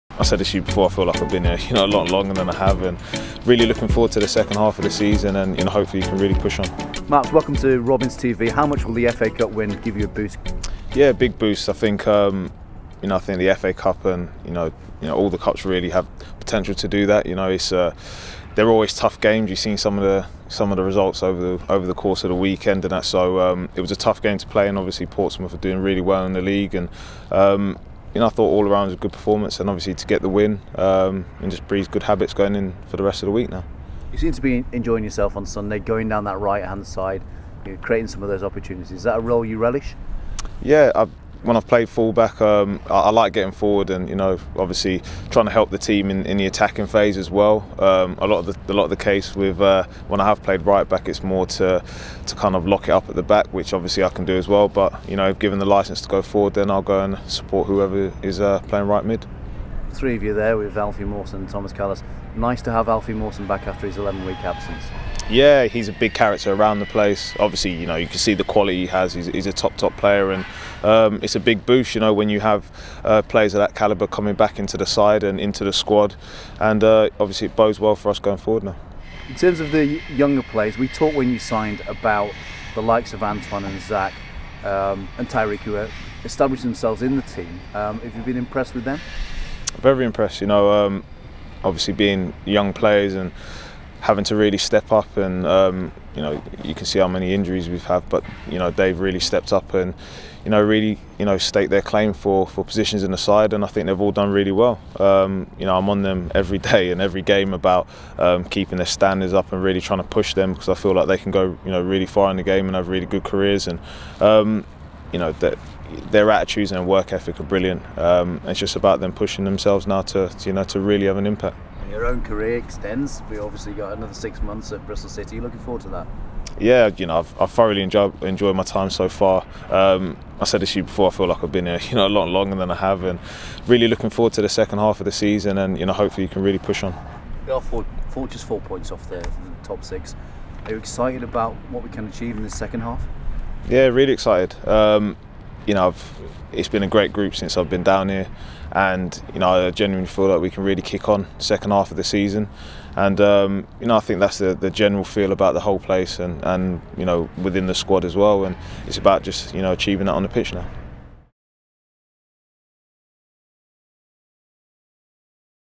INTERVIEW | Mariappa relishing second-half of season!
Adrian Mariappa is relishing life in BS3 as City embark on the second half of the campaign.

With 168 Premier League appearances and more than 200 in the Championship, the defender joined the Robins in November with an impressive CV – and plenty of fuel in the tank to add to that portfolio with a contact extension until the end of the season.

And he showed that engine in Sunday’s Emirates FA Cup Third Round victory over Portsmouth with forrays from right-back while Tomas Kalas and the returning Alfie Mawson marshalled the middle of the back four.